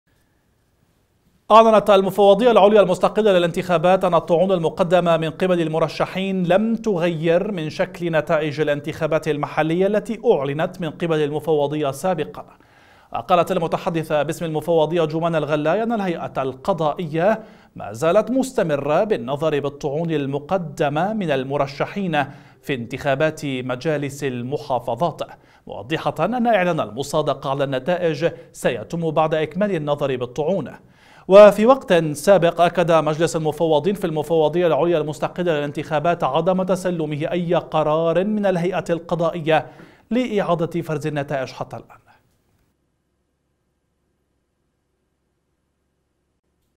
المفوضية: الطعون لم تغيّر نتائج الانتخابات
أعلنت المفوضية العليا المستقلة للانتخابات أن الطعون المقدمة من قبل المرشحين لم تغير من شكل نتائج الانتخابات المحلية التي اعلنت من قبل المفوضية سابقا، وقالت المتحدثة باسم المفوضية جمانة الغلاي ان الهيئة القضائية ما زالت مستمرة بالنظر بالطعون المقدمة من المرشحين في انتخابات مجالس المحافظات؛ موضحة أن اعلان المصادقة على النتائج سيتم بعد اكمال النظر بالطعون، وفي وقت سابق أكد مجلس المفوضين في المفوضية العليا المستقلة للانتخابات عدم تسلمه أي قرار من الهيئة القضائية لإعادة فرز النتائج حتى الآن.